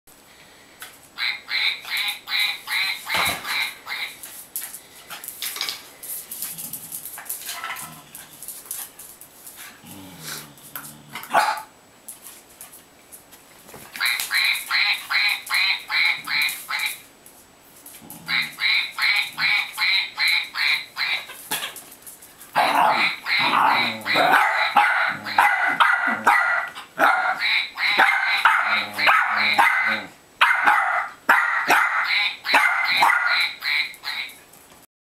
JAKE & LESTER Whose Duck is this anyway? 1-23-2011 002.AVI
Lester my 16 yr old Wirehaired Dachshund let Jake, my 7 month old Connemara Jack Russell Terrier, know in that the Quacking Duck is his in no uncertain terms.